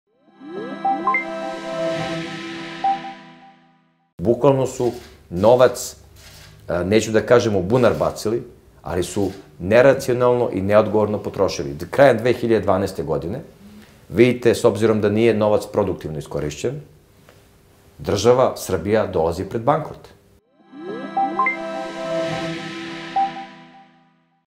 Ministar finansija Siniša Mali: Srbija je 2012. godine bila na ivici bakrota!
Ministar finansija u Vladi Republike Srbije Siniša Mali otkrio je malo poznate informacije o ekonomskoj politici bivše vlasti i kako je Srbija došla na ivicu bankrota.